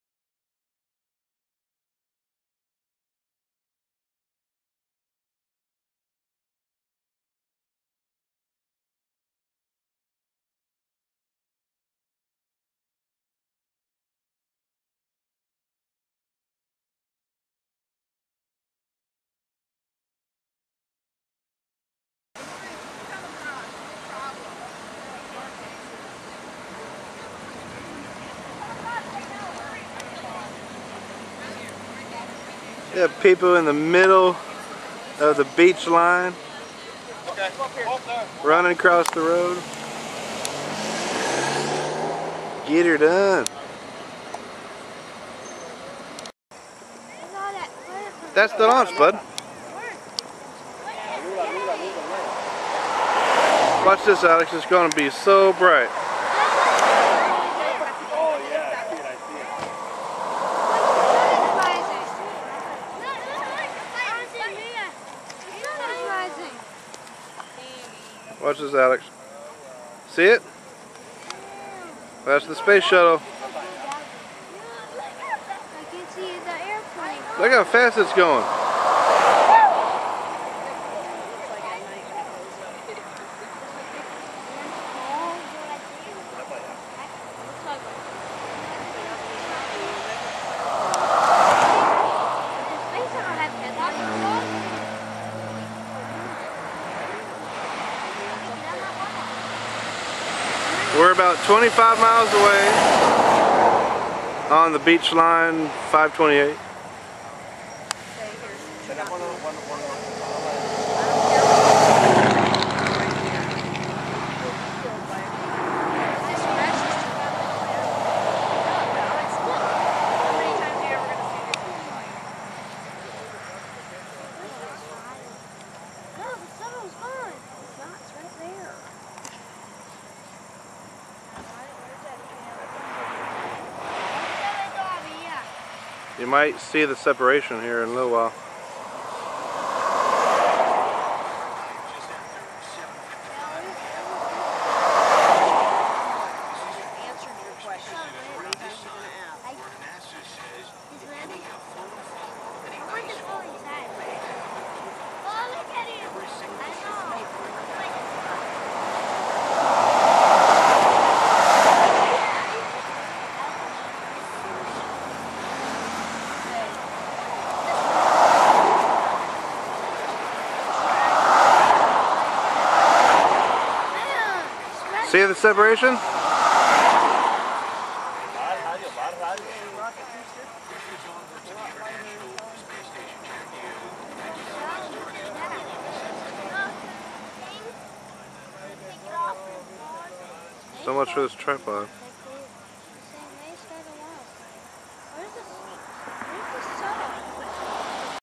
Space Shuttle Endeavour Launch w/ separation of S.R.B's!
11/14/08

About 15 miles away from the launch pad on Highway 528 near Titusville.